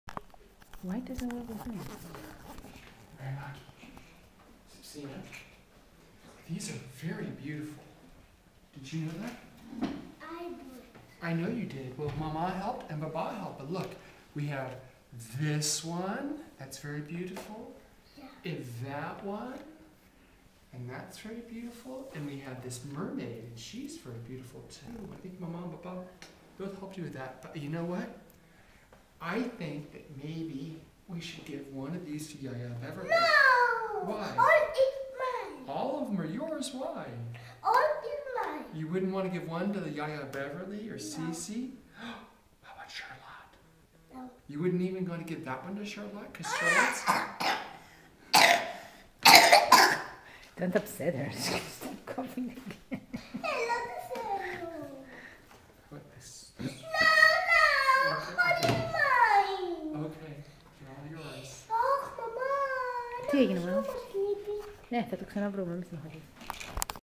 Marina's View on Sharing Her Artwork
Marina thinks her artworks should be hanging only on her wall for her own personal viewing pleasure.  She learned how to share from one of her Aunts!